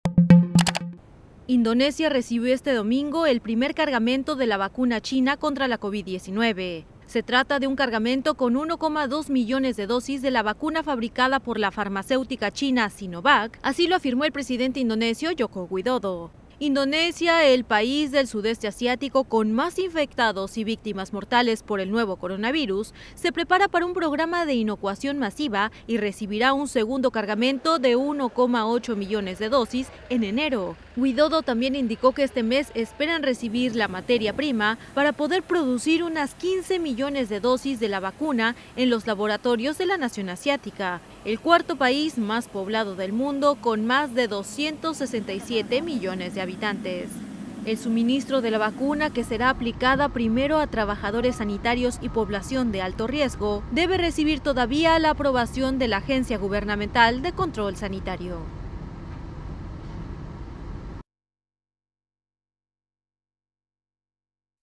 Indonesia recibe el primer cargamento de la vacuna Sinovac contra la covid-19
Yakarta (Indonesia), 7 dic (EFE).- Indonesia, el país del Sudeste Asiático con más infectados y víctimas mortales por el nuevo coronavirus, ha recibido un primer cargamento con 1,2 millones de dosis de la vacuna fabricada por la farmacéutica china Sinovac contra la covid-19, informó el presidente, Joko Widodo.
Imagen: Presidencia de Indonesia, sólo uso informativo.